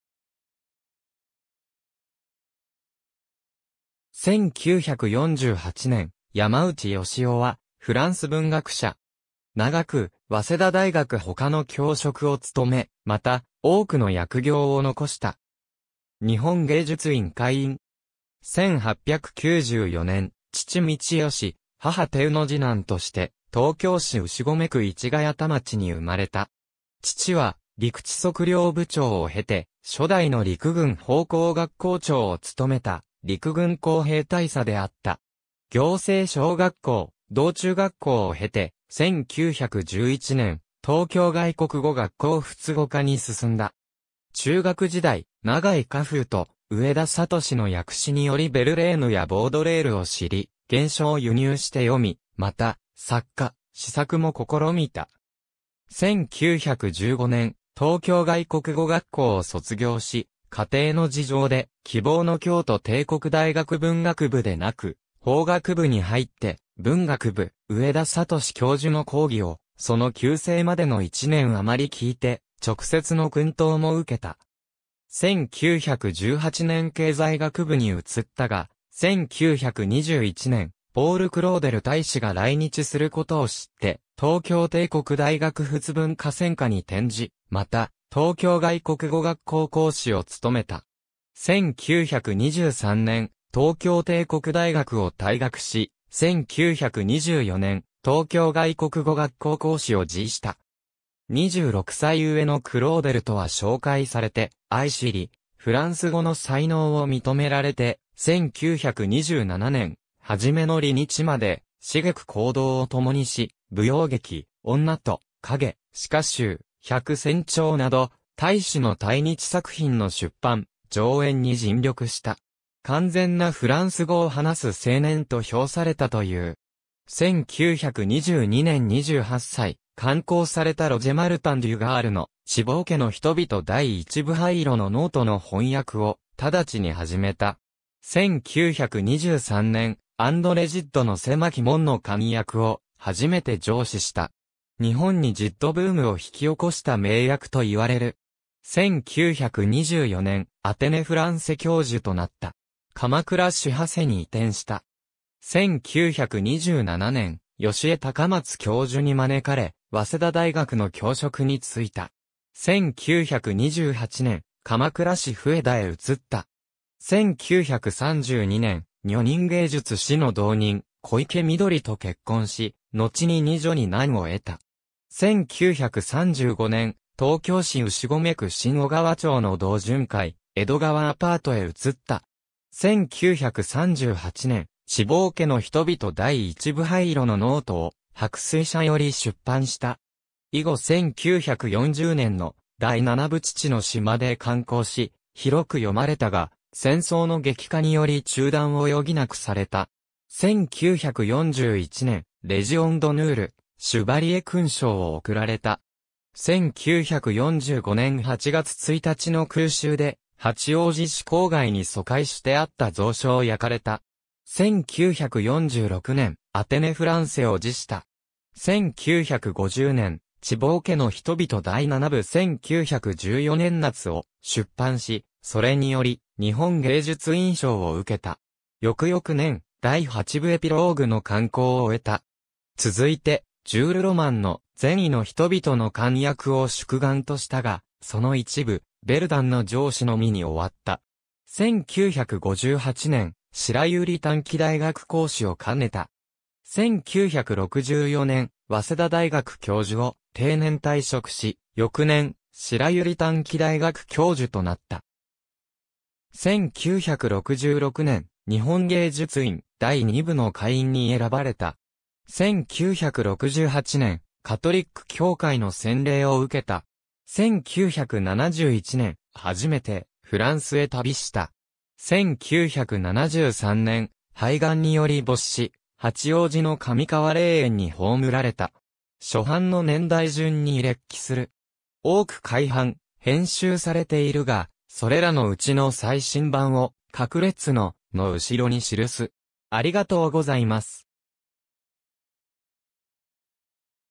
山内義雄
1948年 山内 義雄（やまのうち よしお、1894年3月22日 - 1973年12月17日）は、フランス文学者。
長く早稲田大学ほかの教職を務め、また、多くの訳業を遺した。
日本芸術院会員。
1894年（明治27年）、父通義、母テウの次男として、東京市牛込区（現在の東京都新宿区）市谷田町に生まれた。
父は陸地測量部長を経て初代の陸軍砲工学校長を務めた陸軍工兵大佐であった。
暁星小学校・同中学校を経て、1911年（17歳）、東京外国語学校仏語科に進んだ。
中学時代、永井荷風と上田敏の訳詩によりヴェルレーヌやボードレールを知り、原書を輸入して読み、また、作歌・詩作も試みた。
1915年（大正4年）（21歳）、東京外国語学校を卒業し、 家庭の事情で、希望の京都帝国大学文学部でなく、法学部に入って、文学部上田敏教授の講義をその急逝までの1年余聴いて、直接の薫陶も受けた。
1918年経済学部に移ったが、1921年、ポール・クローデル大使が来日することを知って東京帝国大学仏文科専科に転じ、また、東京外国語学校講師を務めた。
1923年、東京帝国大学を退学し、1924年、東京外国語学校講師を辞した。
26歳上のクローデルとは紹介されて相知り、フランス語の才能を認められて、1927年初めの離日まで繁く行動を共にし、舞踊劇『女と影』（1922年、帝国劇場）、詞華集『百扇帖』（1927年、新潮社）など、大使の滞日作品の出版、上演に尽力した。
「完全なフランス語を話す青年」と評されたと言う。
1922年（大正11年）28歳、刊行されたロジェ・マルタン・デュ・ガールの『チボー家の人々 第1部 灰色のノート』の翻訳をただちに始めた。
1923年、アンドレ・ジッドの狭き門の完訳を初めて上梓した。
日本にジッド・ブームを引き起こした名訳と言われる。
1924年（30歳）、アテネ・フランセ教授となった。
鎌倉市長谷に移転した。
1927年、吉江喬松教授に招かれ、早稲田大学の教職に就いた。
1928年、鎌倉市笛田へ移った。
1932年（38歳）、『女人芸術』誌の同人、小池みどりと結婚し、のちに2女2男を得た。
1935年、東京市牛込区（現在の東京都新宿区）新小川町の同潤会江戸川アパートへ移った。
1938年（44歳）、『チボー家の人々 第一部 灰色のノート』を白水社より出版した。
以後1940年の『第7部 父の死』まで刊行し、広く読まれたが、戦争の激化により中断を余儀なくされた。
1941年、レジオン・ドヌール、シュヴァリエ勲章を贈られた。
1945年8月1日の空襲で、八王子市郊外に疎開してあった蔵書を焼かれた。
1946年、アテネ・フランセを辞した。
1950年（56歳）、『チボー家の人々 第7部 1914年夏』を出版し、それにより日本芸術院賞を受けた。
翌々年『第8部 エピローグ』の刊行を終えた。
続いて、ジュール・ロマンの『善意の人々』の完訳を宿願としたが、その一部『ヴェルダン』の上梓のみに終わった。
1958年（64歳）、白百合短期大学講師を兼ねた。
1964年（70歳）、早稲田大学教授を定年退職し、翌年、白百合短期大学教授となった。
1966年、日本芸術院第2部の会員に選ばれた。
1968年、カトリック教会の洗礼を受けた。
1971年（77歳）、初めてフランスへ旅した。
1973年（昭和48年）（79歳）、肺がんにより没し、八王子の上川霊園に葬られた。
初版の年代順に列記する。
多く改版・編集されているが、それらのうちの最新版を、各列の「 / 」の後ろに記す。